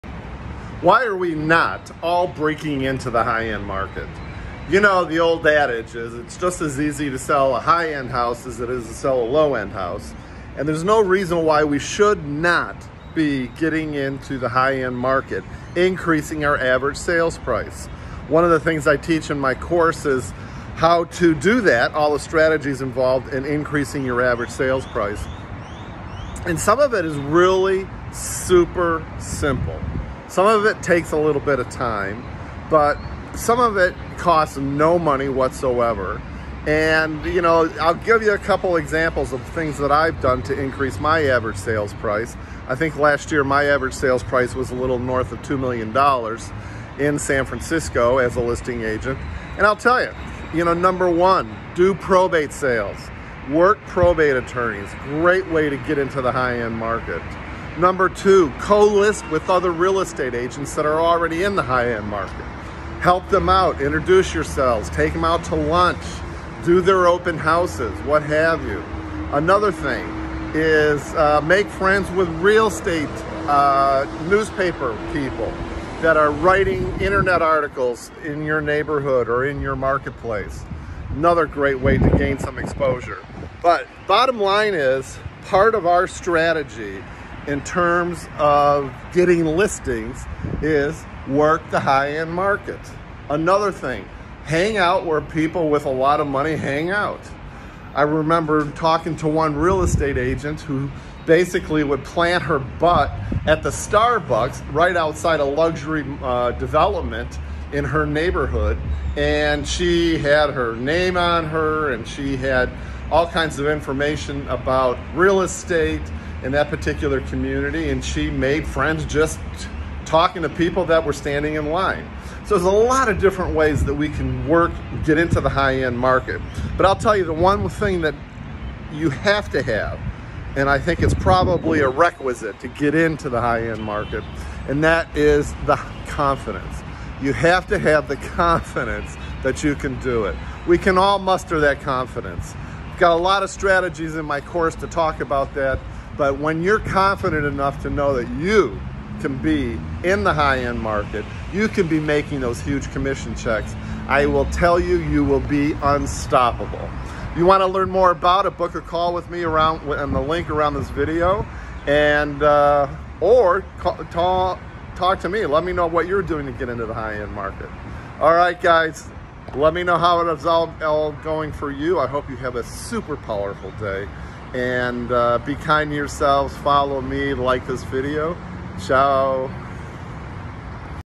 Why not break into the high-end market?
Realtors, want an easy way to get more high-end listings and more commission income using a very simple marketing technique?  Then use the "Micro Momentum Marketing Technique" to get more listings!